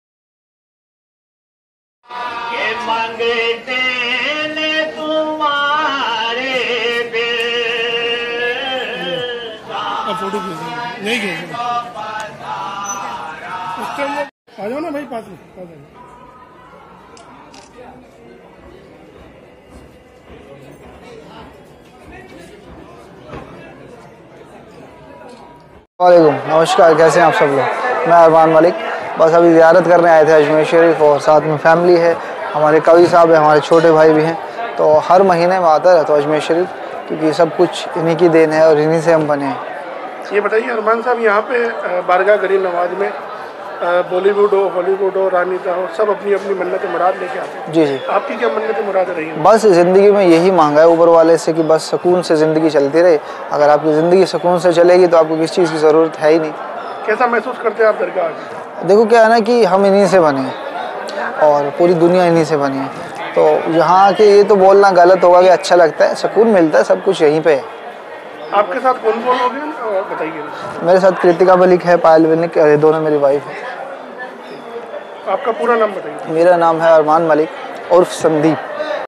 सोशल मीडिया कलाकार अरमान मलिक व उनकी दोनो पत्निया पायल और कीर्तिका ने ख्वाजा साहब की TS NEWS AJMER
सोशल मीडिया कलाकार अरमान मलिक व उनकी दोनो पत्निया पायल और कीर्तिका ने ख्वाजा साहब की दरगाह मे की ज़ियारत ।

अजमेर: महान सूफ़ी संत हज़रत ख्वाजा मोइनुद्दीन चिश्ती की दरगाह मे सोशल मीडिया के जाने माने कलाकार अरमान मलिक व उनकी दोनो पत्निया कीर्तिका व पायल ने ख्वाजा साहब की मज़ार पर मखमली चादर व अकीदत के फूल पेश कर कामयाबी के लिए दुआ की अरमान मलिक व उनकी दोनो पत्नियों को ज़ियारत दरगाह के खादिम सैयद कवि मोहम्मद चिश्ती ने कराई। ज़ियारत के बाद अरमान मलिक के दस्तारबंदी की व उनकी पत्नियों को दुपट्टा उड़ाया, और दरगाह का तबर्रुख भेंट किया । बतचित के दोरान अरमान मलिक ने बताया की वह हमेशा ही ख्वाजा साहब की दरगाह मे आते रहते है और इस दरगाह से पूरी दुनिया भर से आये ज़ायरीनो की मुराद पूरी होती है और सभी पर ख्वाजा साहब का आशीर्वाद बना रहता है इस दर पर आने के बाद बोहोत सुकून मेहसूस होता है।

बाइट - अरमान मलिक, सोशल मीडिया कलाकार